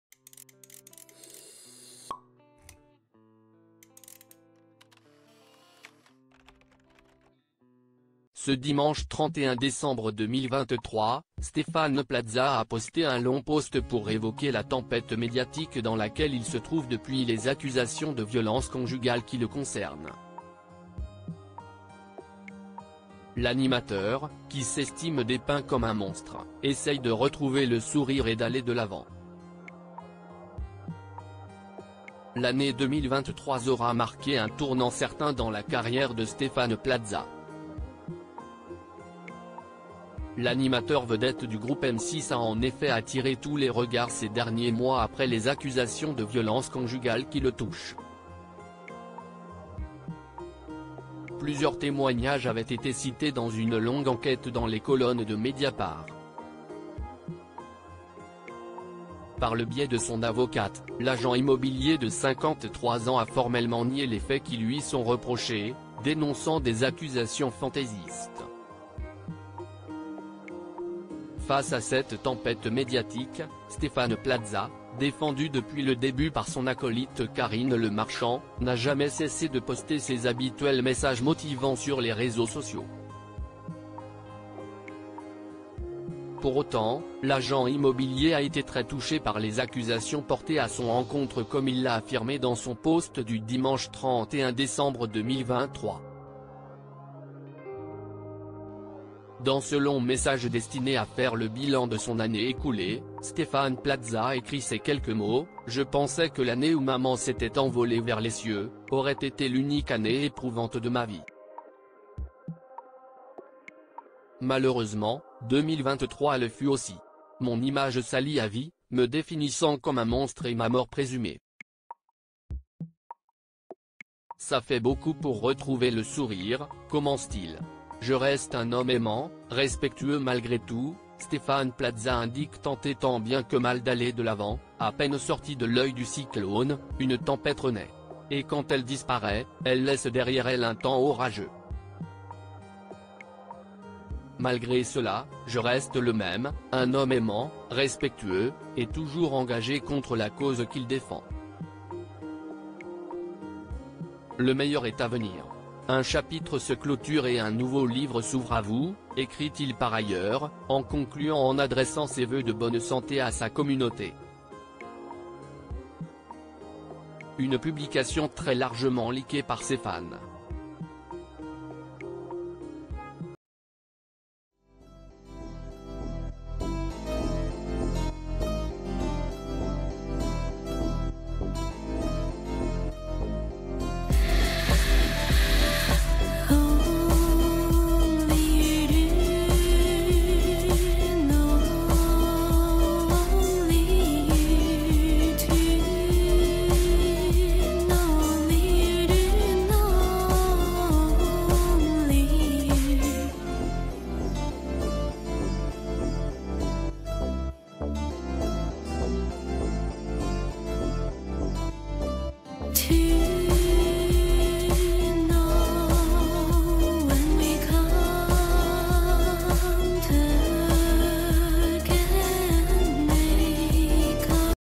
“Mon image salie à vie” Stéphane Plaza évoque sa “mort présumée” depuis les accusations de violenc
“Mon image salie à vie” : Stéphane Plaza évoque sa “mort présumée” depuis les accusations de violences conjugales